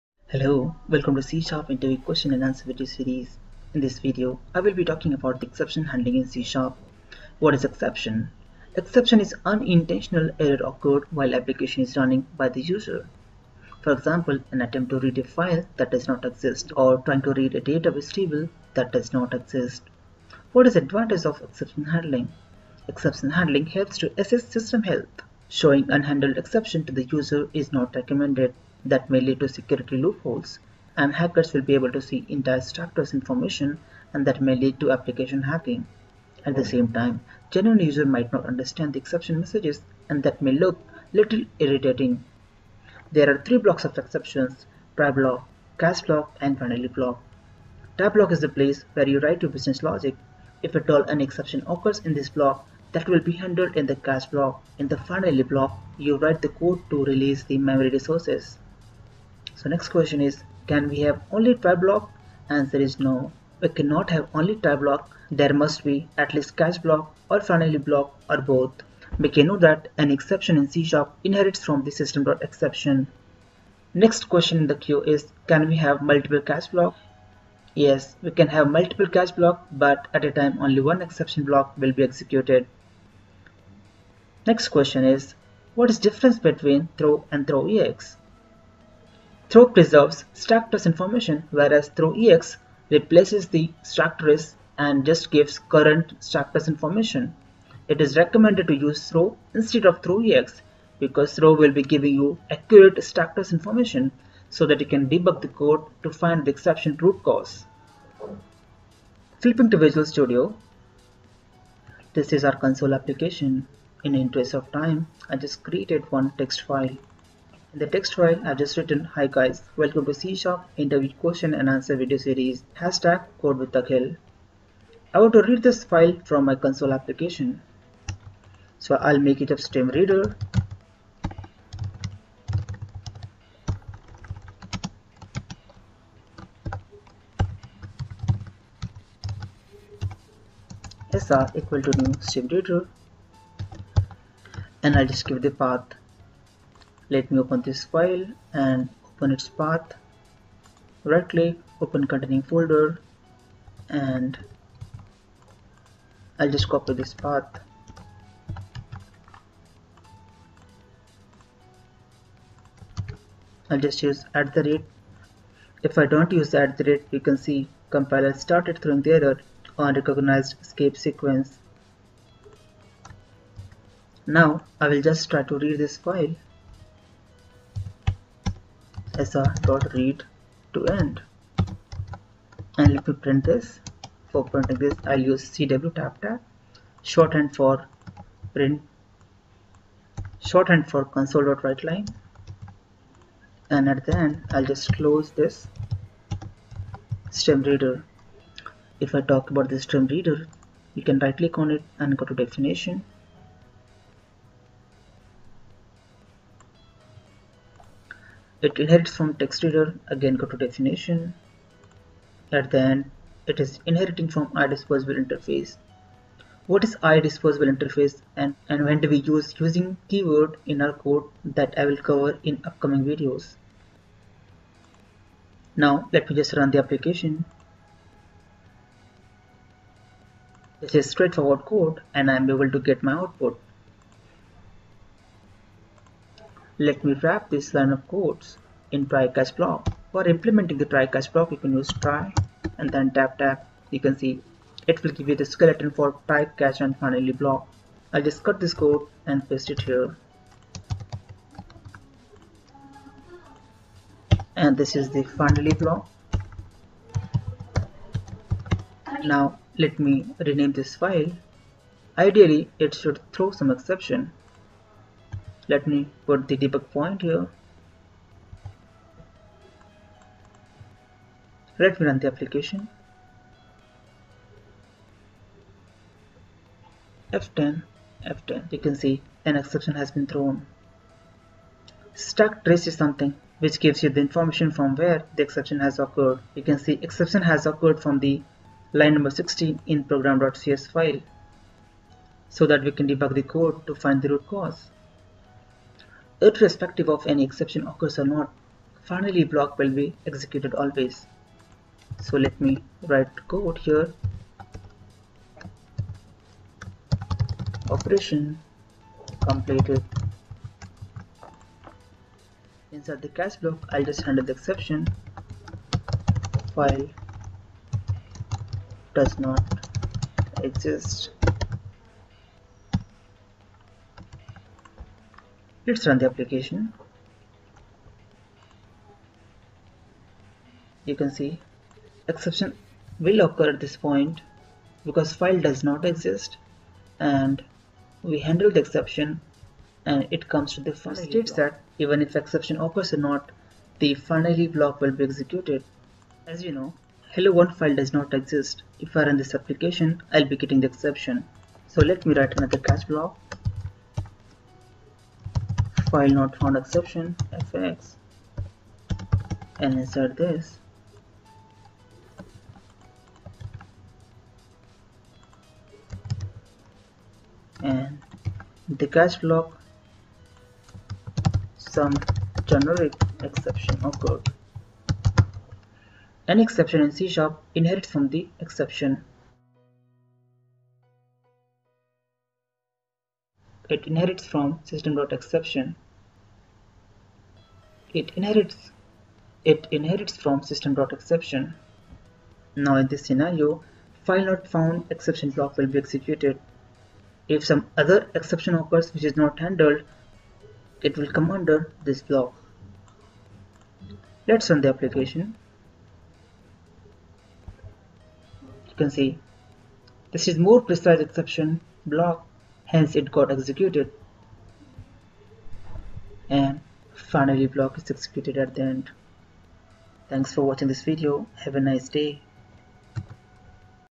Exception handling in C#
What is Exception?
An exception is a problem that arises during the execution of a program.

try  - This block holds the code that may throw an exception.
catch - This block catches the exception thrown by the try block.
finally - This block holds the default code.
throw - This block is Used to throw an exception manually.

NOTE : finally block is always executed
    try block can have multiple catch blocks but at a time only one catch block is executed
    try block cant be implimented alone, it must have corresponding catch or Finally block

Difference between "throw" vs "throw ex"
throw ex resets the stack trace (so your errors would appear to originate from HandleException)
throw doesn't - the original offender would be preserved.